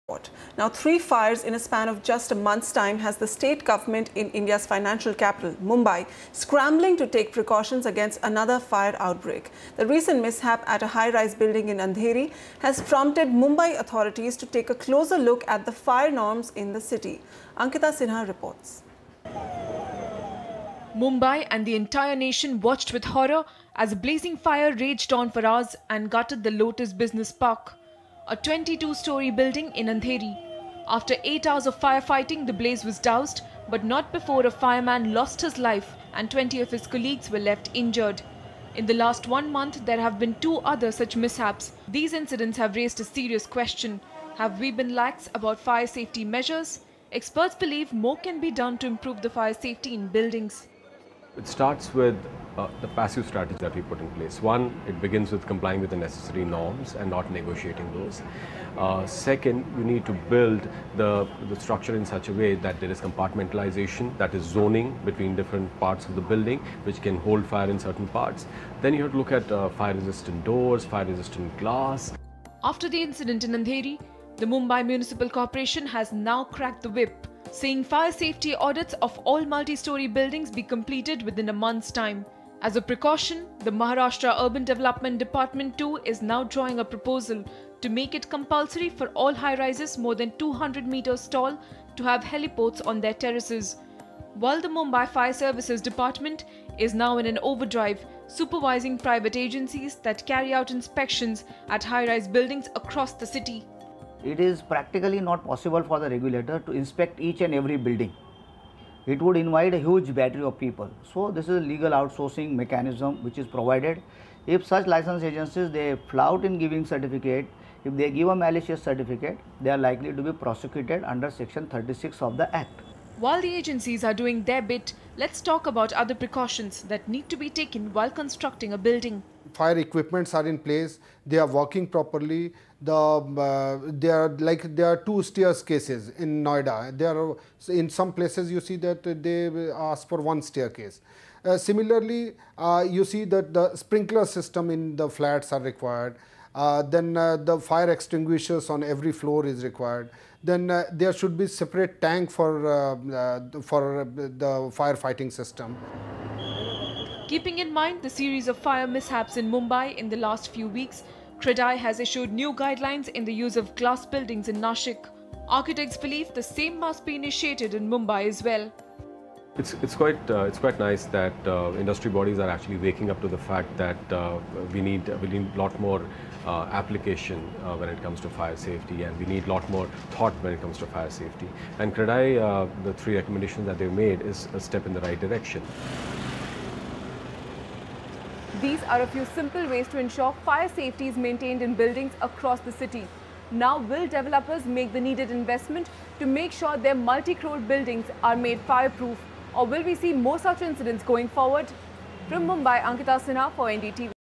Mumbai: Making buildings fire resistant
In the light of recent incidents of building fires in Mumbai, Ankita Sinha brings you this report which gets to the bottom of reasons and remedies.